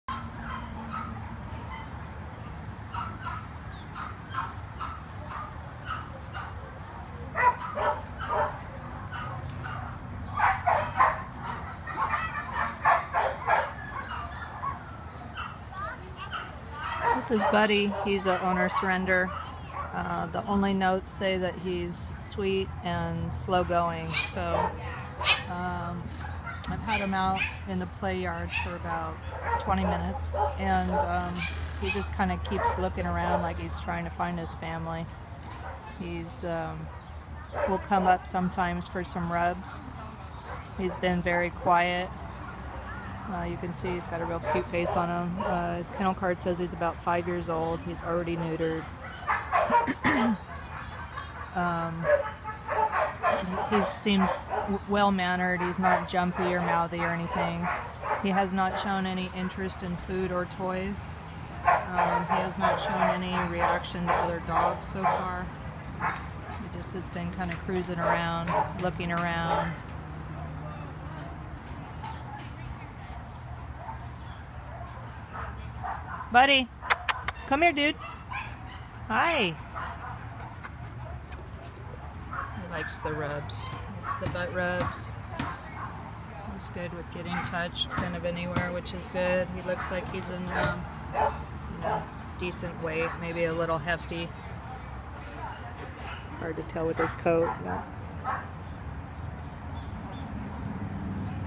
Owner surrender Buddy at OC 7/7/15
BUDDY
Pet ID: A0675179
Sex: N
Age: 5 Years
Color: TAN
Breed: AUST SHEPHERD - MIX
Kennel: 362
Notes say sweet and slow going-
Volunteer notes , he seems well mannered (not jumpy or mouthy) , quiet, sweet, likes rubs, no dog reaction, no interest in food or toys.
Oc shelter orange California